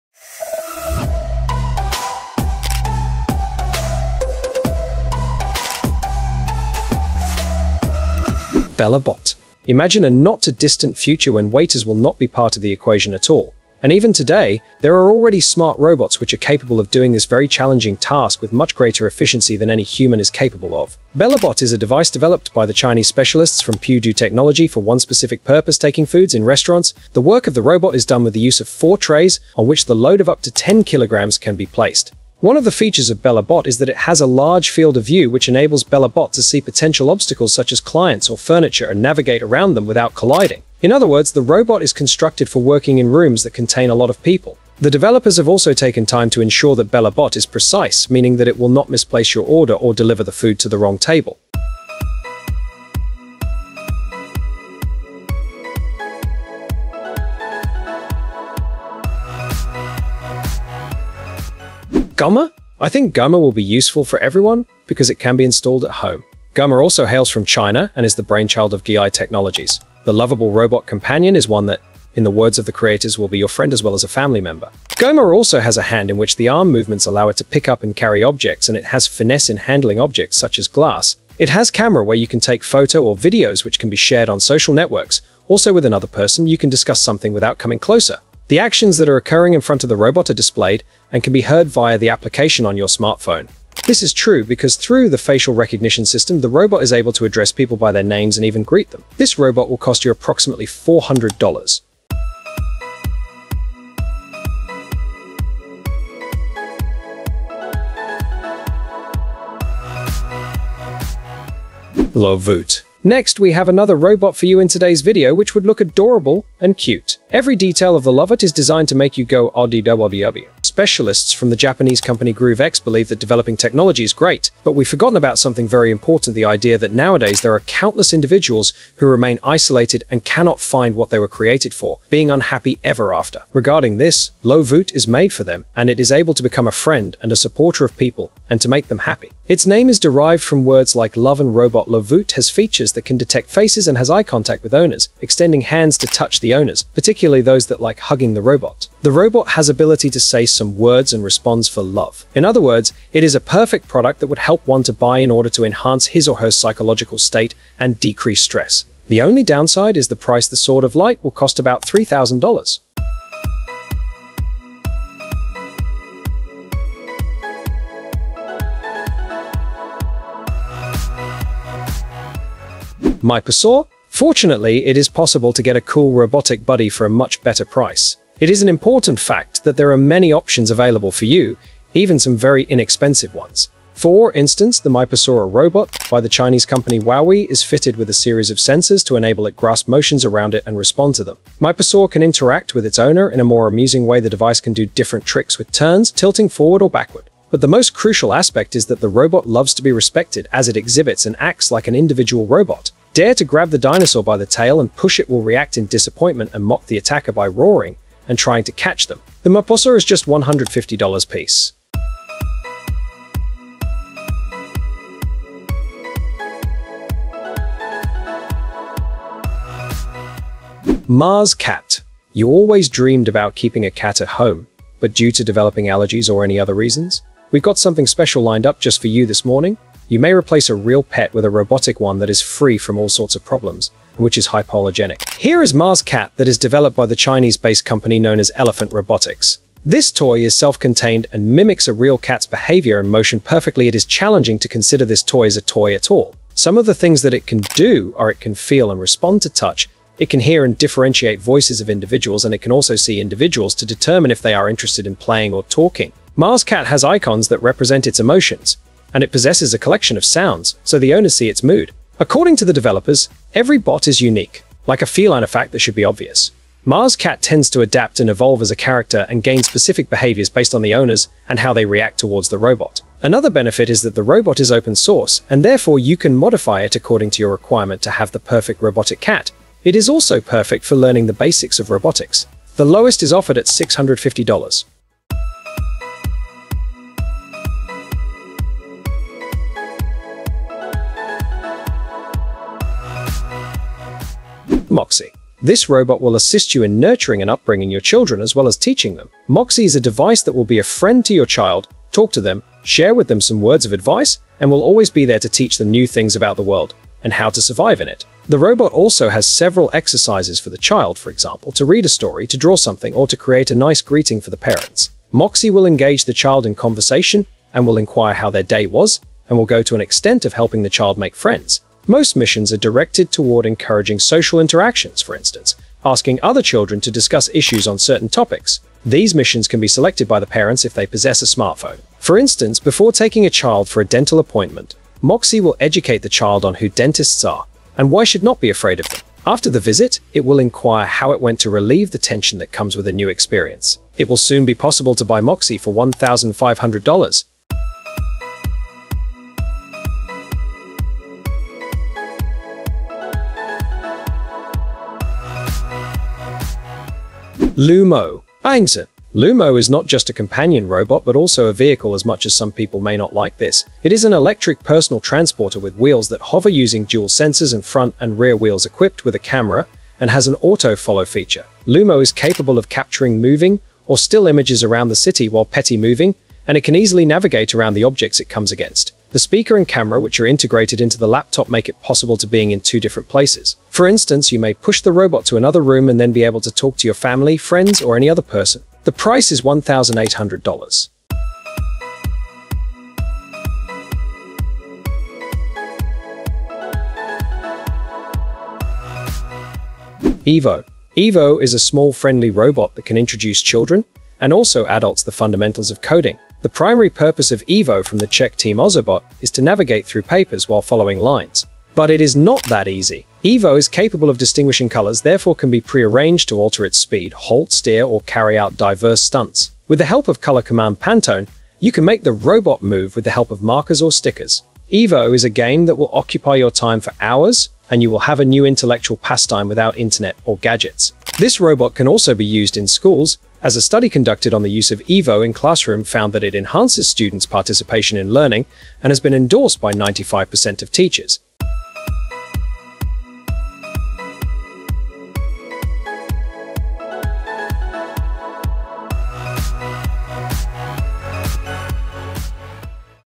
Top 7 MODERN Robotic Gadgets You Can BUY in 2024
Top 7 MODERN Robotic Gadgets You Can BUY in 2024. Welcome to the Future of Gadgets! In this video, we explore the top 7 modern Robotic gadgets you can buy in 2024. From high-tech robots to innovative companions, discover the cutting-edge technology that's shaping our world. Whether you're a tech enthusiast or just curious about what's next, this video is a must-watch!

Meet the Robots:
Bellot - The efficient restaurant robot
GMA - The lovable home companion
Lovo - The adorable friend for the lonely
Miposaur - The interactive dinosaur robot
Mars Cat - The robotic feline friend
Moxy - The educational robot for kids
Lumo - The electric personal transporter
Evo - The coding robot for all ages